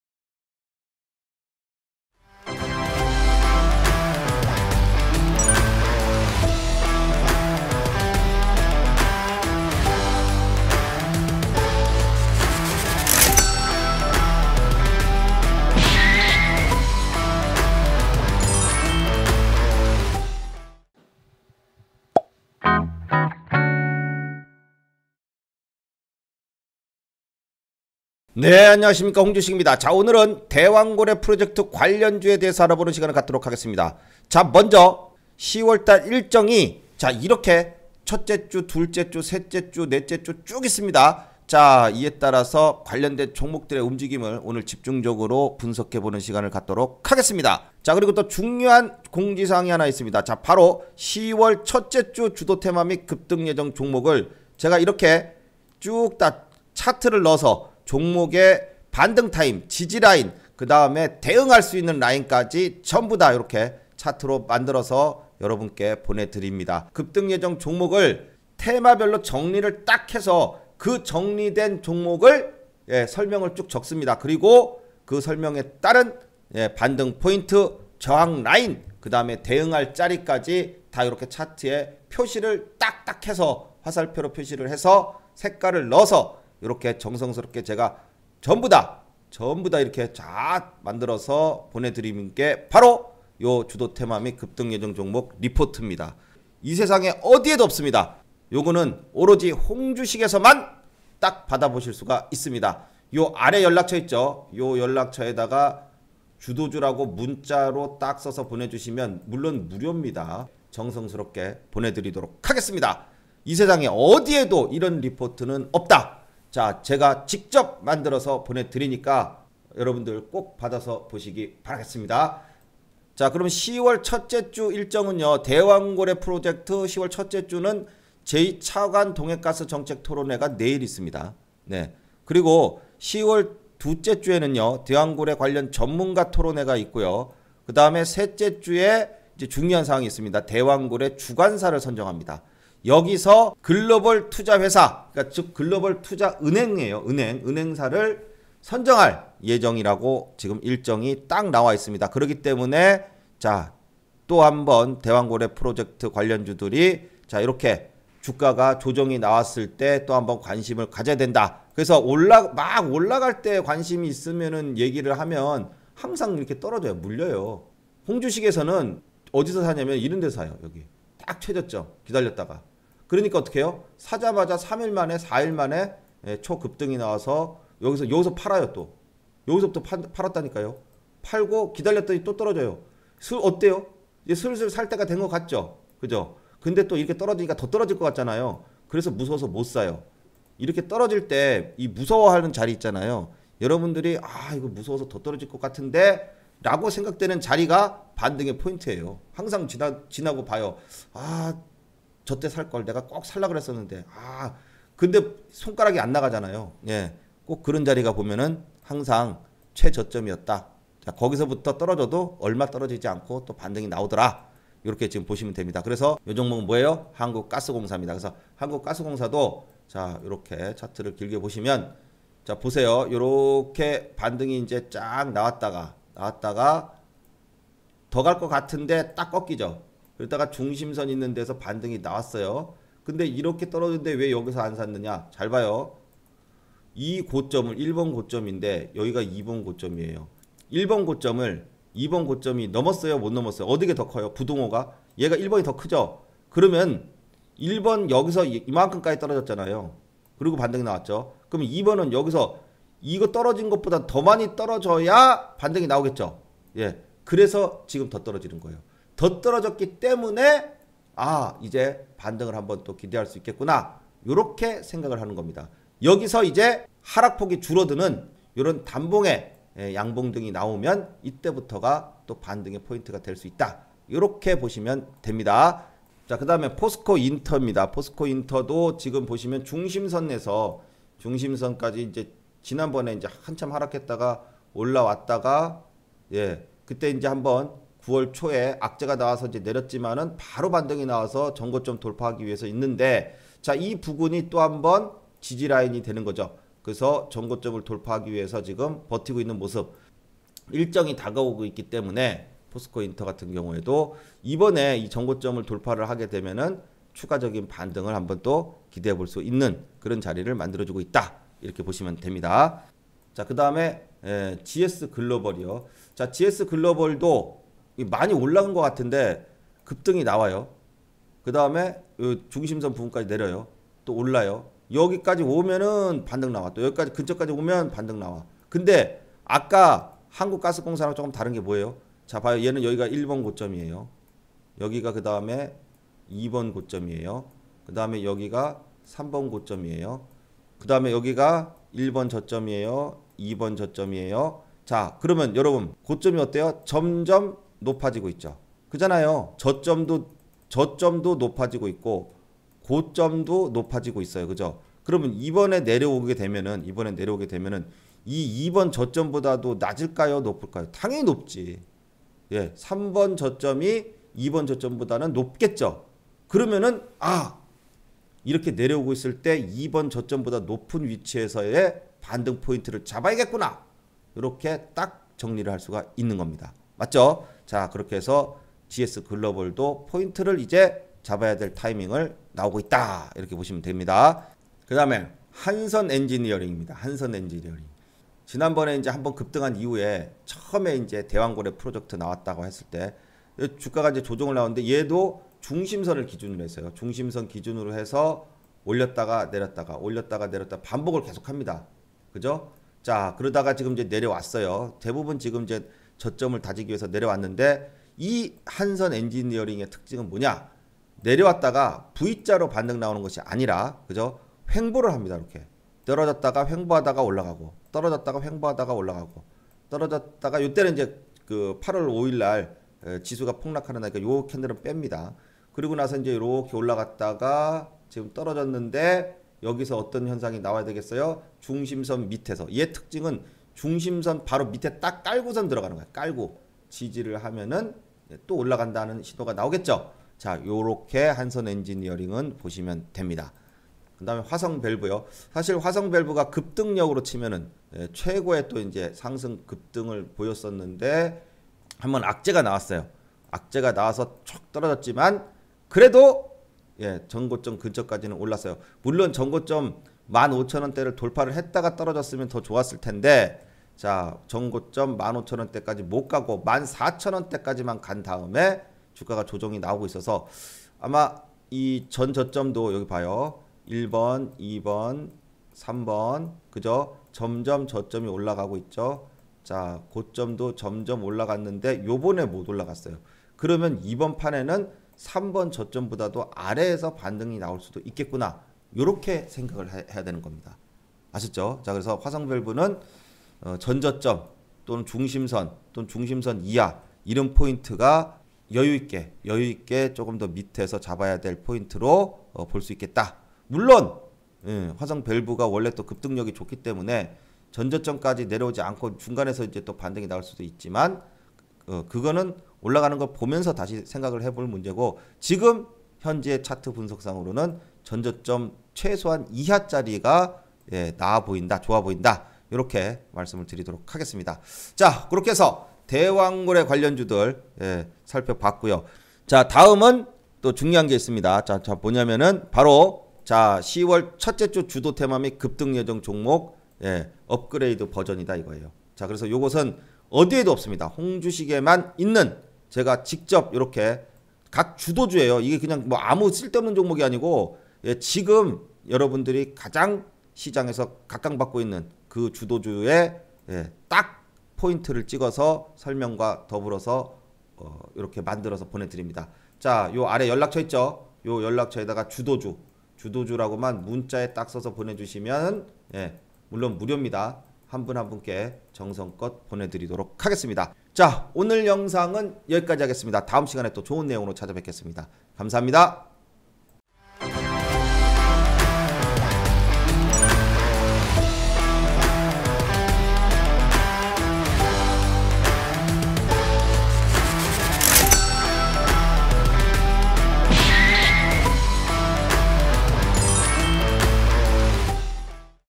대왕고래 관련주 한국가스공사 포스코인터내셔널 GS글로벌 화성밸브의 반등자리 분석
● 홍주식 주도테마 리포트
→ "주도주" 보내주세요

대왕고래 관련주
내일부터 주목해야 할 종목